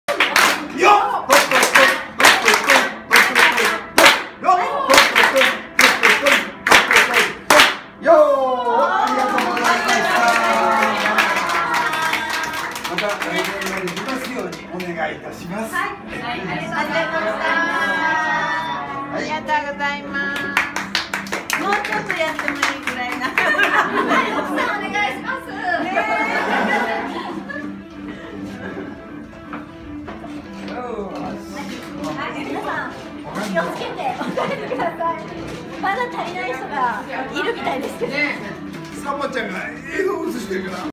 頑張ります！
男の店の従業員一同 気持ちを１つにして 明日へ向かって頑張ります！息のあったチームワークの練習のため 手拍子合わせてみました。